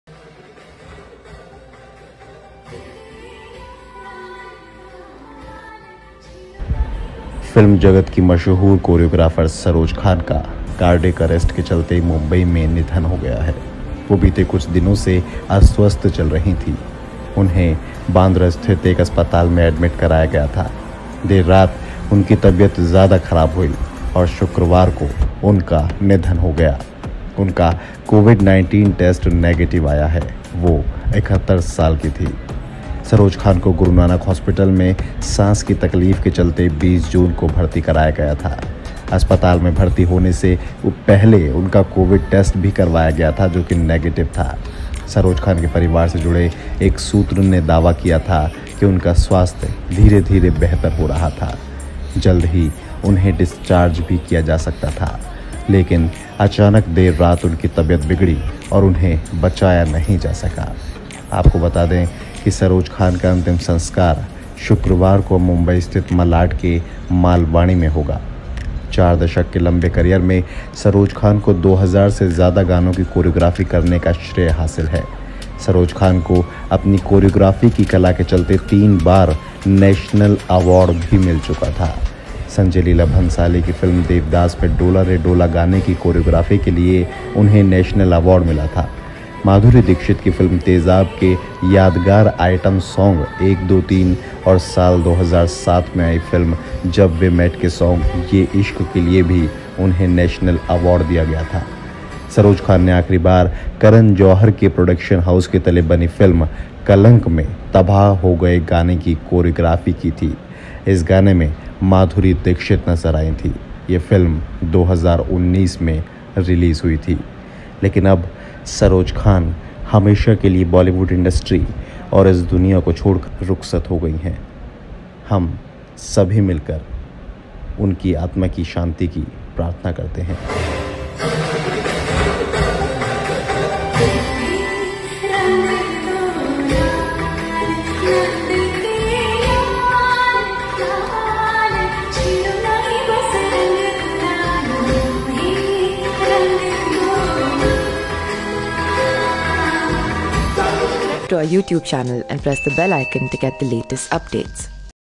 RIP Saroj Khan | तीन बार जीता National Award, इस वजह से थी ख़ास, अब ली अंतिम साँस | Quick News
RIPSaroj Khan | तीन बार जीता National Award, इस वजह से थी ख़ास, अब ली अंतिम साँस